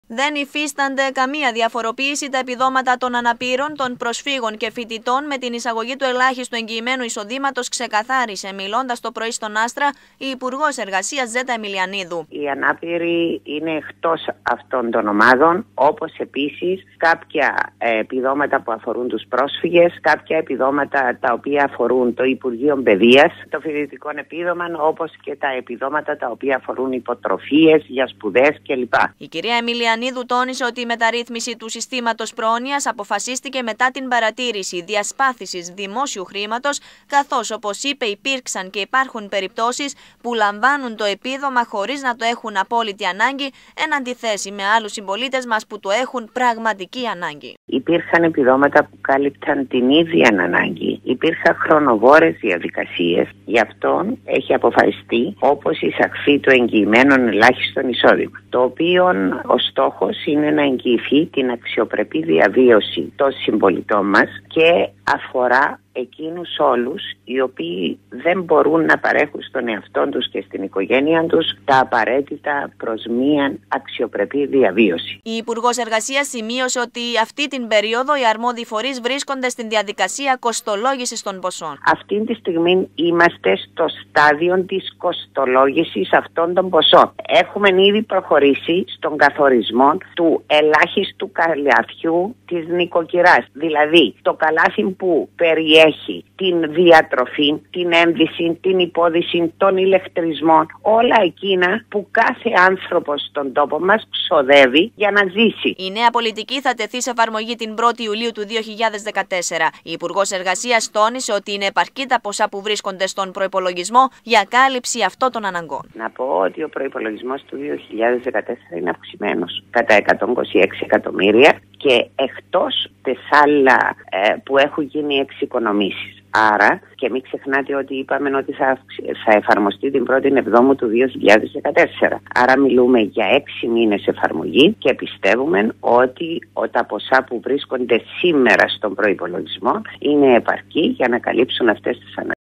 Η Υπ.Εργασίας μιλά για το ελάχιστο εγγυημένο εισόδημα
Η αλλαγή στο σύστημα πρόνοιας αποφασίστηκε καθώς υπήρχε διασπάθιση χρήματος και δεν υπήρχε στόχευση, δήλωσε στον ΑΣΤΡΑ  η υπουργός εργασίας.
 Σημείωσε ότι υπήρχαν χρονοβόρες διαδικασίες αλλά και περιπτώσεις που τα επιδόματα λαμβάνονται από άτομα που δεν τα έχουν ανάγκη.
 Η κυρία Αιμιλιανίδου εξήγησε ότι γι αυτό αποφασίστηκε η εισαγωγή του ελάχιστου εγγυημένου εισοδήματος το οποίο αφορά εκείνους που δεν μπορούν να εξασφαλίσουν τα προς το ζειν και που τα εισοδήματα τους είναι κάτω από το όριο της αξιοπρεπούς διαβίωσης.
 Εξήγησε ότι στο πλαίσιο αυτής της πολιτικής δεν διαφοροποιούνται τα επιδόματα για ανάπηρους, για πρόσφυγες και για την παιδεία.
 Πρόσθεσε ότι θα γίνει συγκέντρωση της παροχής των επιδομάτων μόνο από το υπουργείο εργασίας και αυτό είπε, έγινε με τον προϋπολογισμό του 2014 με στόχο την πρώτη εβδόμου 2014 να αρχίσει να εφαρμόζεται η νέα πολιτική.
 Η υπουργός εργασίας ανέφερε ότι σε αυτή τη φάση το όλο εγχείρημα βρίσκεται στη διαδικασία κοστολόγησης των ποσών.